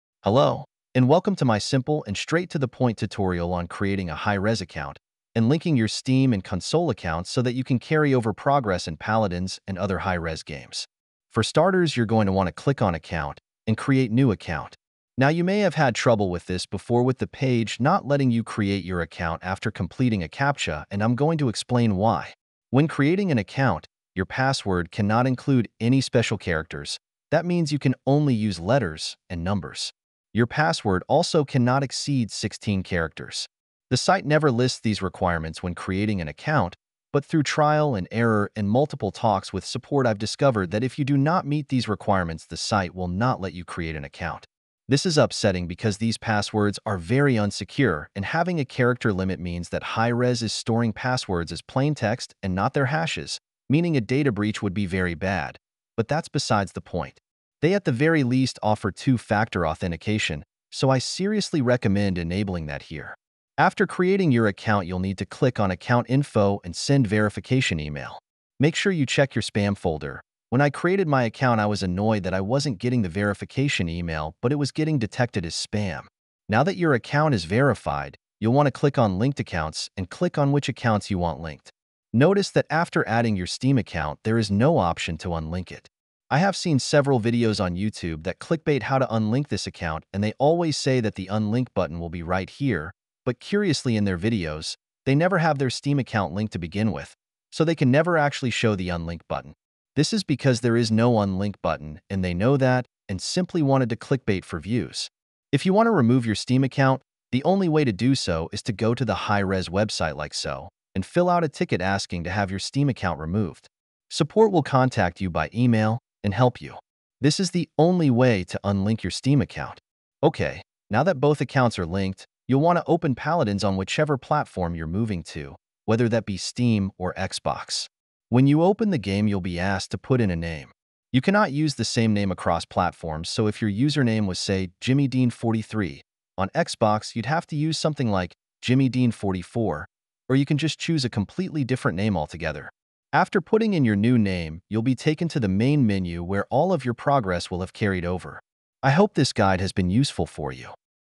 How to make a Hi-Rez account, syncing your Paladins data, and how to unlink your Steam account
This guide will help you to create your Hi-Rez account. I have seen multiple outdated or outright incorrect guides on how to create an account. This guide will also show you how to sync your data between the PC and Console version of Paladins, along with how you can actually unlink your Steam account, since multiple videos outright lie and clickbait about how to do it.